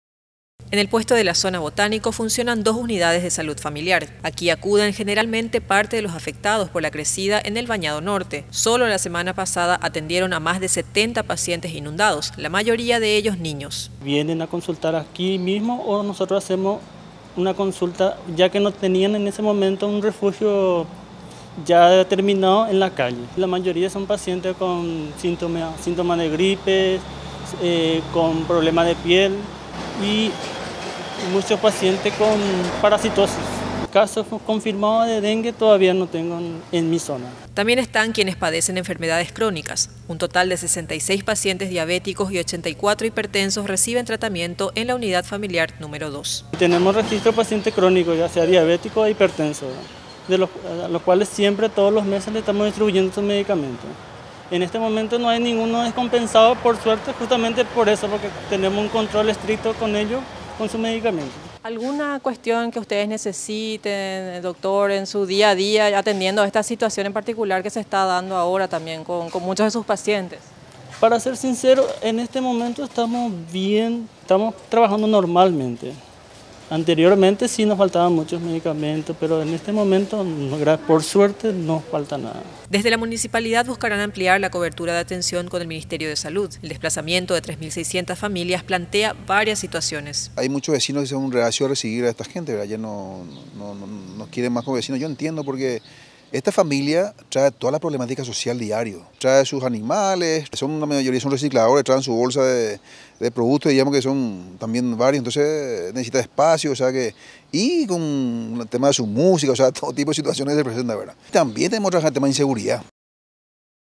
Problemas respiratorios y de piel aquejan a inundados
Más de 3.600 familias ya tuvieron que abandonar sus viviendas por la crecida del río Paraguay. A este drama se suman los problemas de salud propios de esta situación: Hay muchos casos de enfermedades respiratorias y de piel, sobre todo en niños.